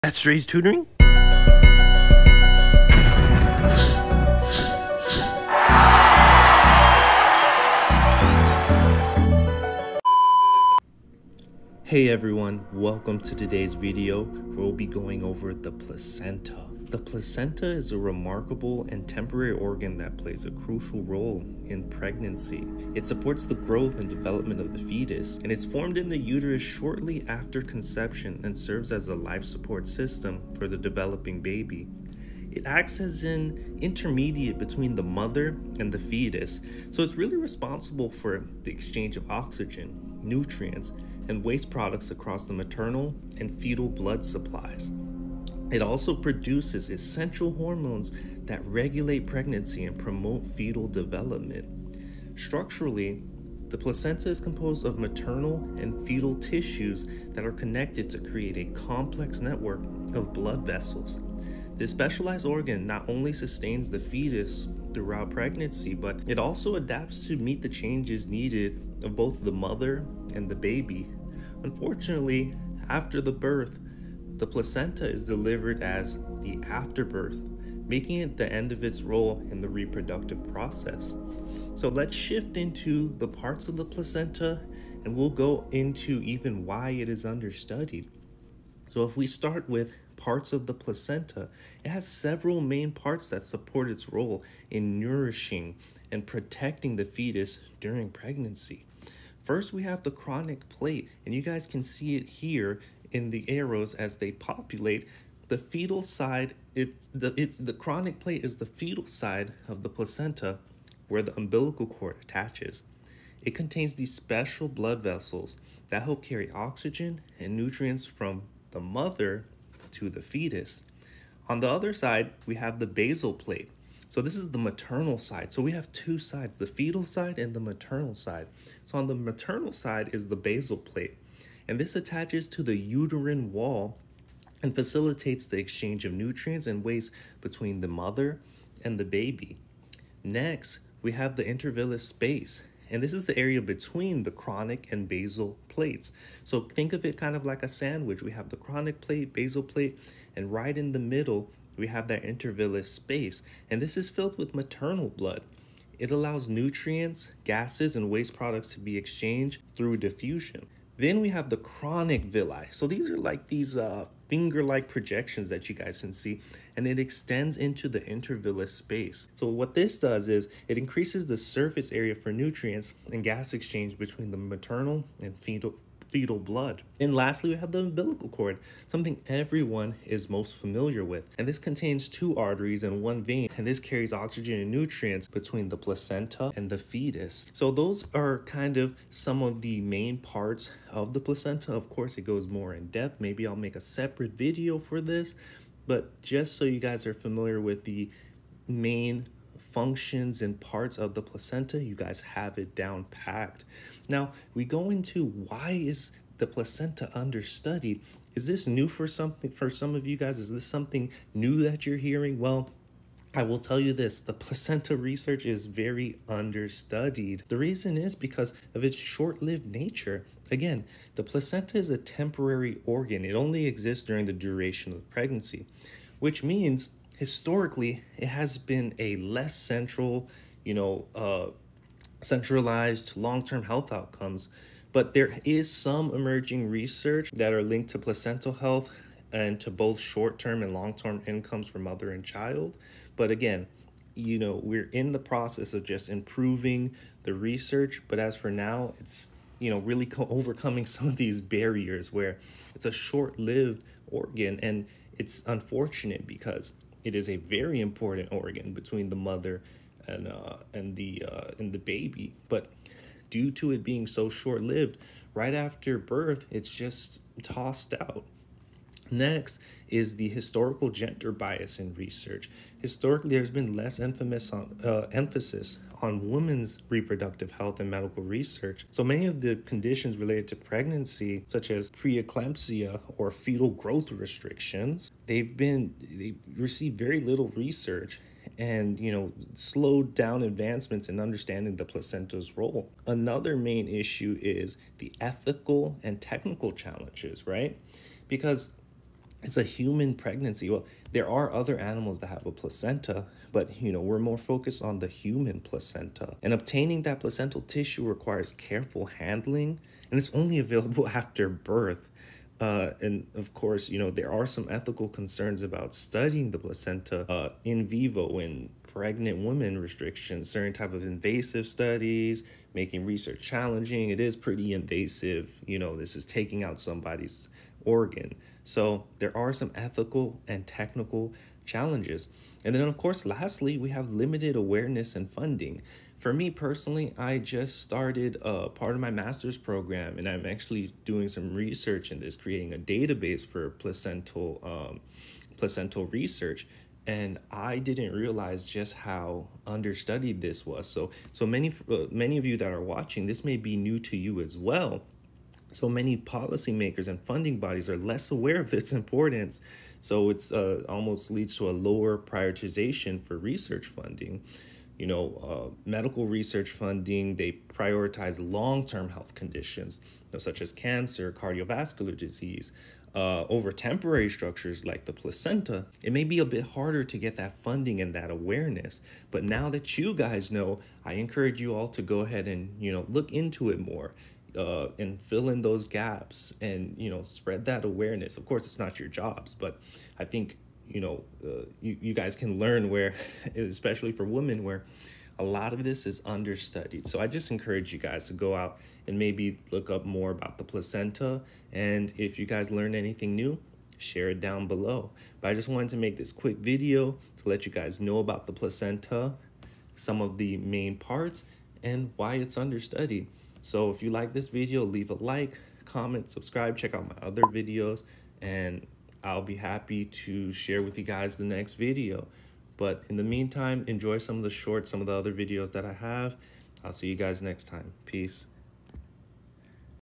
The Placenta Is Understudied: Quick Overview
Hey guys! Welcome to the video where we will be going over the understudied organ, the placenta.

📖Books:
A Paradoxical Life Where Did We Come From?

Your place for early learning in STEM